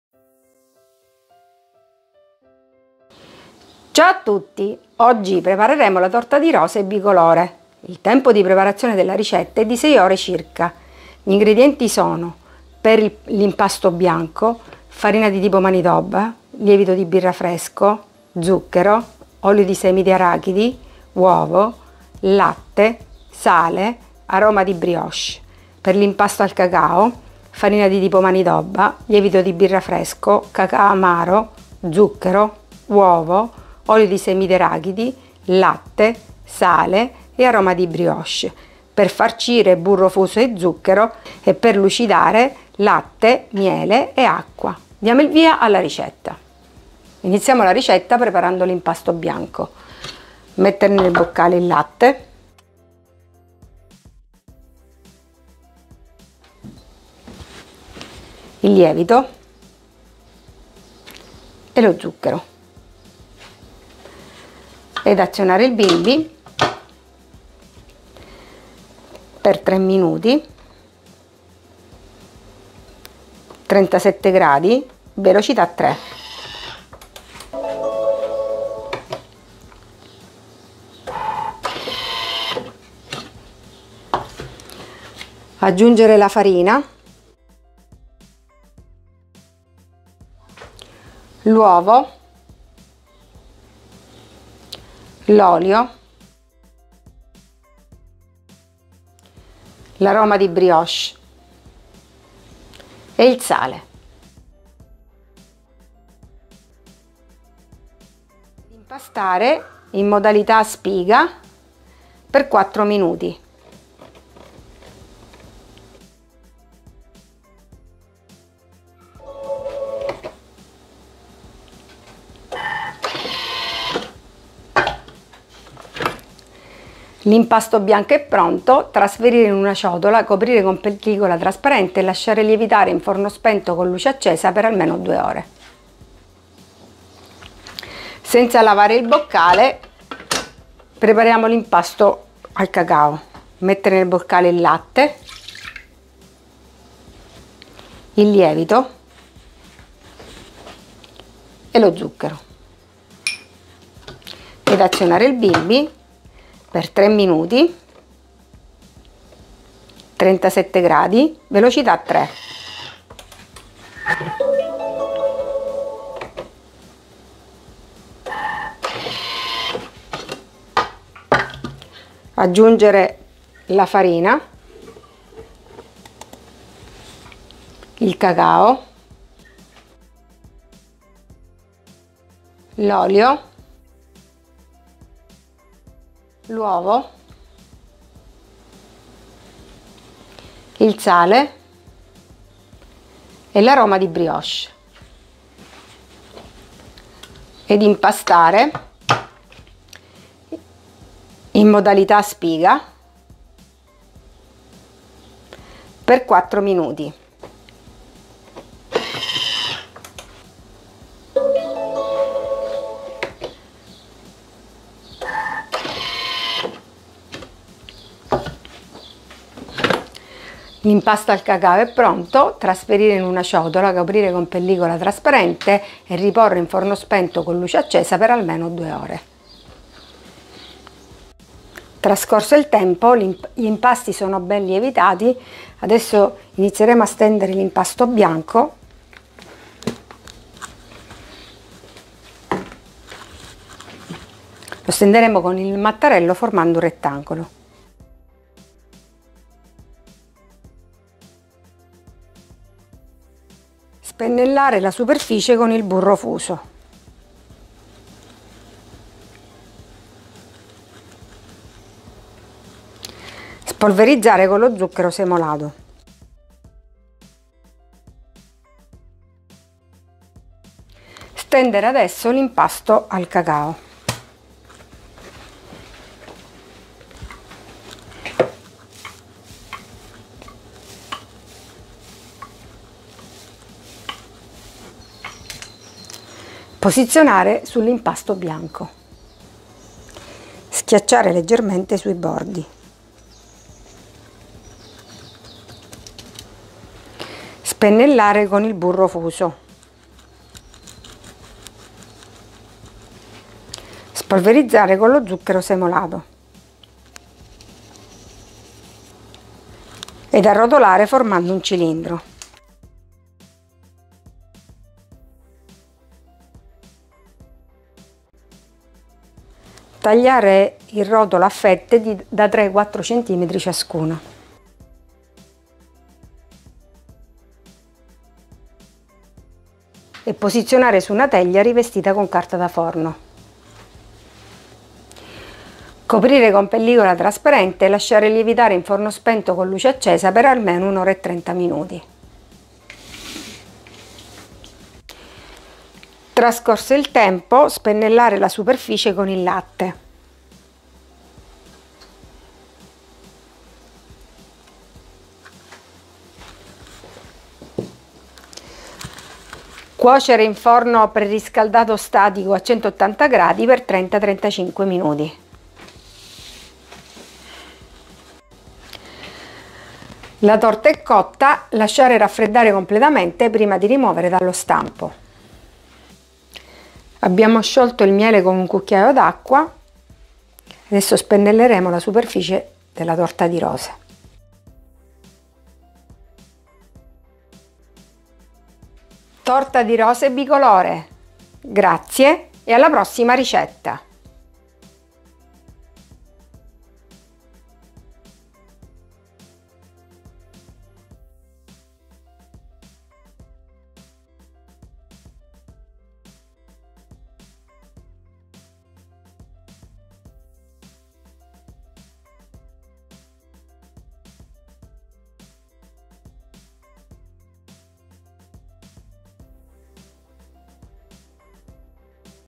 Torta di rose bicolore per bimby TM6 TM5 TM31 TM21
Ingredienti per l'impasto bianco:
330 g di farina di tipo manitoba
8 g di lievito di birra fresco
70 g di zucchero
50 g di olio di semi di arachidi
1 uovo
100 g di latte
mezzo cucchiaino di sale
10 gocce di aroma di brioche

Ingredienti per l'impasto al cacao:
300 g di farina di tipo manitoba
8 g di lievito di birra fresco
30 g di cacao amaro
70 g di zucchero
1 uovo
50 g di olio di semi di arachidi
100 g di latte
10 gocce di aroma di brioche

Ingredienti per farcire e decorare:
burro fuso q.b.
zucchero q.b.
latte a temperatura ambiente q.b. per spennellare

2 cucchiaini di miele
1 cucchiaio di acqua calda

English
Ingredients for the white dough:
330 grams of Manitoba type flour
8 grams of fresh brewer's yeast
70 grams of sugar
50 grams of peanut seed oil
1 egg
100 grams of milk
half a teaspoon of salt
10 drops of brioche flavoring

Ingredients for the cocoa dough:
300 grams of Manitoba type flour
8 grams of fresh brewer's yeast
30 grams of bitter cocoa
70 grams of sugar
1 egg
50 grams of peanut seed oil
100 grams of milk
10 drops of brioche flavoring

Ingredients for filling and decorating:
Melted butter, as needed
Sugar, as needed
Room temperature milk, for brushing
2 teaspoons of honey
1 tablespoon of warm water

Español
Ingredientes para la masa blanca:
330 g de harina tipo Manitoba
8 g de levadura fresca de cerveza
70 g de azúcar
50 g de aceite de cacahuate
1 huevo
100 g de leche
media cucharadita de sal
10 gotas de aroma de brioche

Ingredientes para la masa de cacao:
300 g de harina tipo Manitoba
8 g de levadura fresca de cerveza
30 g de cacao amargo
70 g de azúcar
1 huevo
50 g de aceite de cacahuate
100 g de leche
10 gotas de aroma de brioche

Ingredientes para rellenar y decorar:
Mantequilla derretida, según sea necesario
Azúcar, según sea necesario
Leche a temperatura ambiente, para pincelar
2 cucharaditas de miel
1 cucharada de agua tibia

Deutsch
Zutaten für den weißen Teig:
330 g Manitoba-Mehl
8 g frische Bierhefe
70 g Zucker
50 g Erdnussöl
1 Ei
100 g Milch
ein halber Teelöffel Salz
10 Tropfen Brioche-Aroma

Zutaten für den Kakaoteig:
300 g Manitoba-Mehl
8 g frische Bierhefe
30 g dunkler Kakao
70 g Zucker
1 Ei
50 g Erdnussöl
100 g Milch
10 Tropfen Brioche-Aroma

Zutaten zum Füllen und Dekorieren:
Geschmolzene Butter nach Bedarf
Zucker nach Bedarf
Raumtemperatur Milch zum Bestreichen
2 Teelöffel Honig
1 Esslöffel warmes Wasser